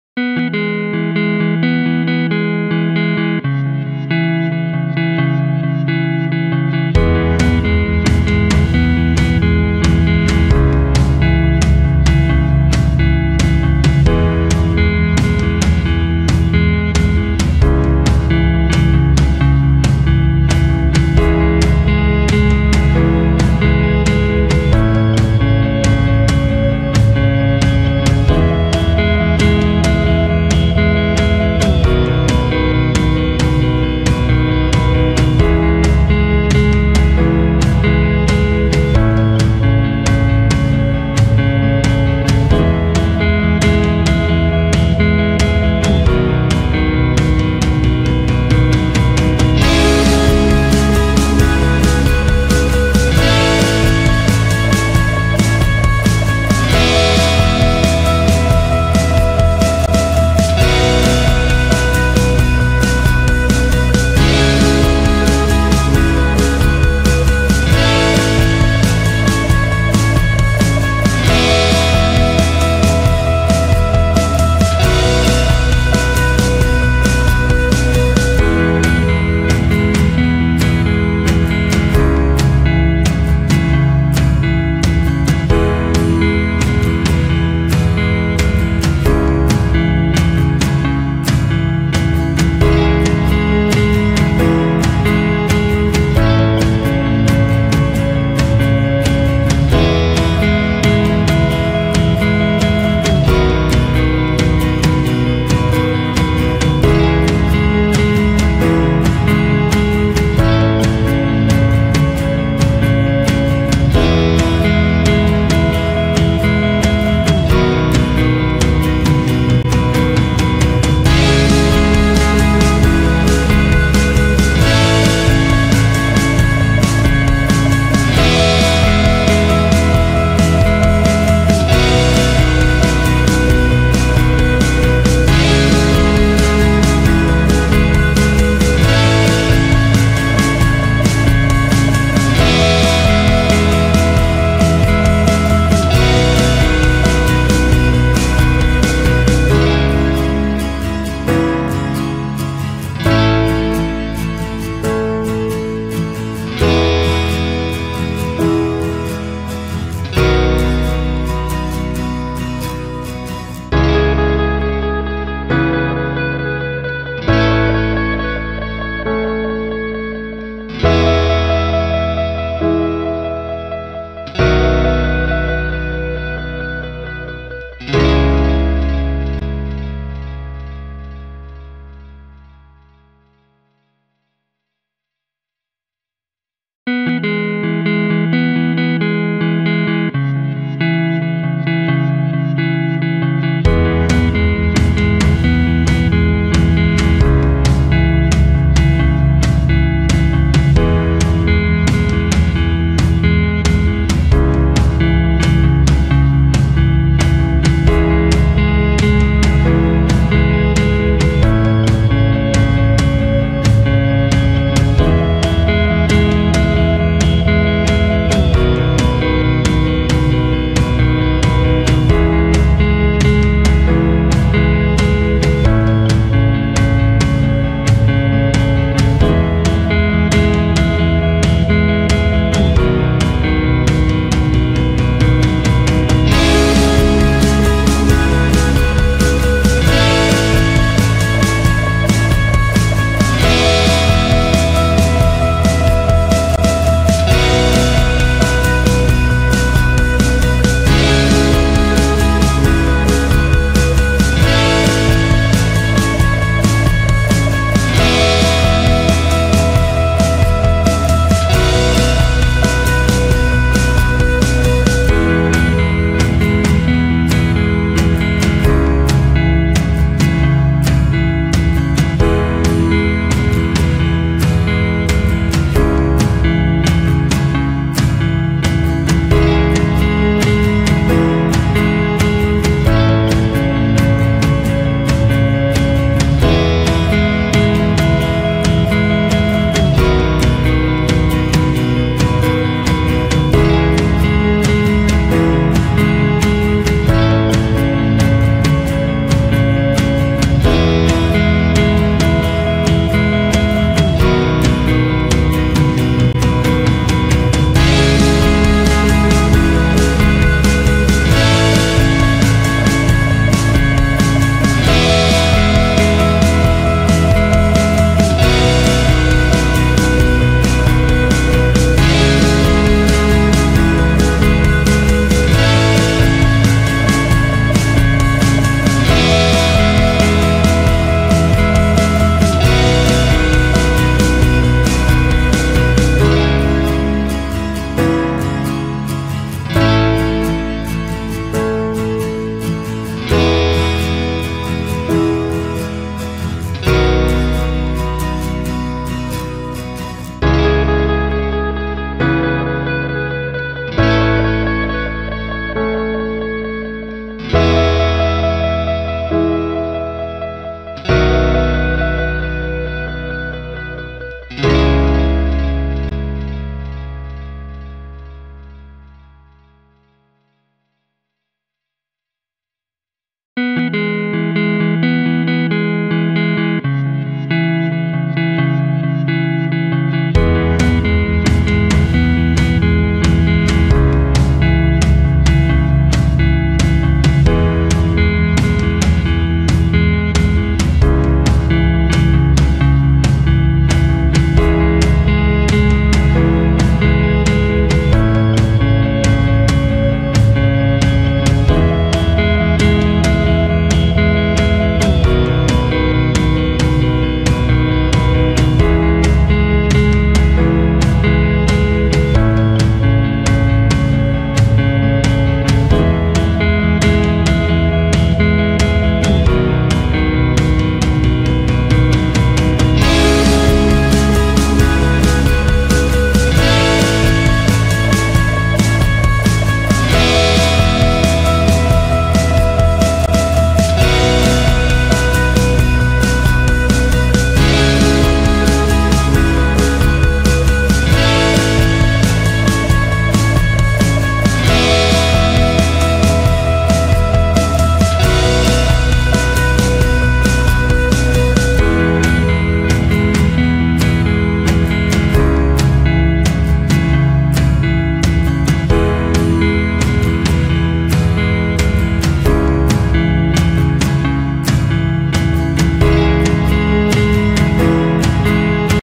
OMokhtar part 2 Color
Timelapse done for (Omar El-Mokhtar) Design Challenge For the Facebook group Arabesque.